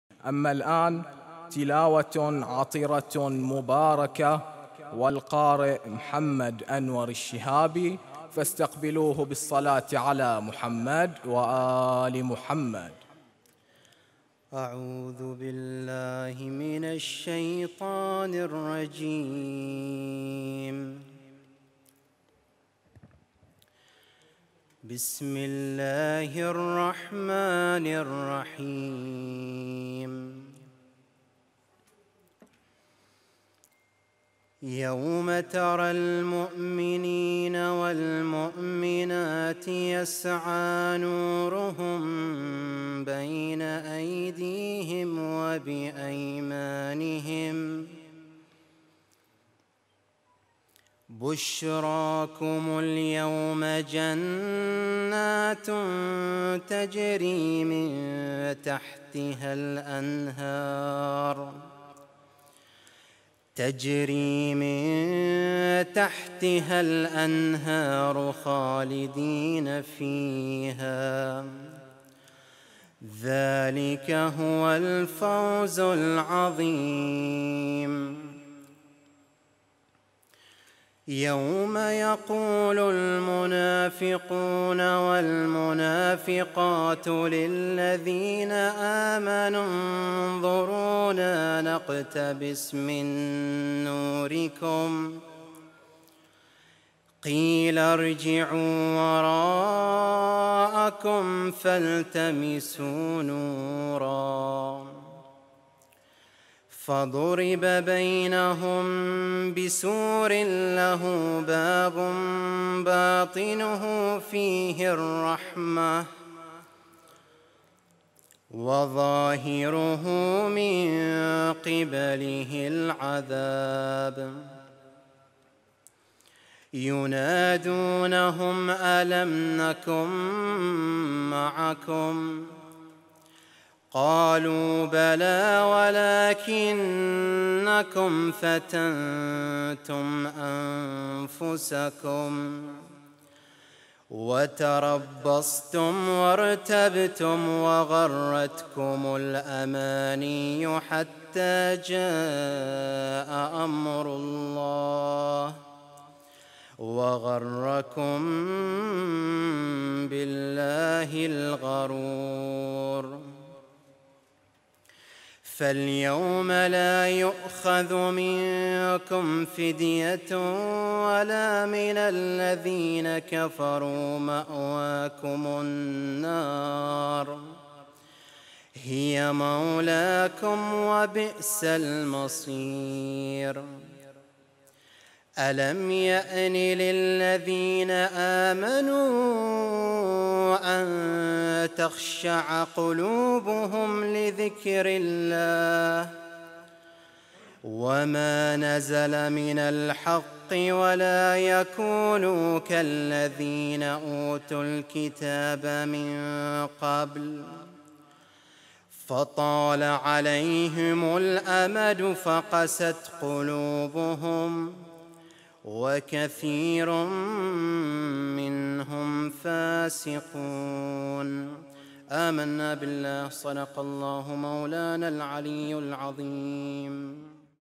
آيات من الذكر الحكيم للقارئ محمد أنور الشهابي - ذكرى ولادة الإمام الجواد(ع)1443هـ
ذكرى ولادة الإمام الجواد(ع)1443هـ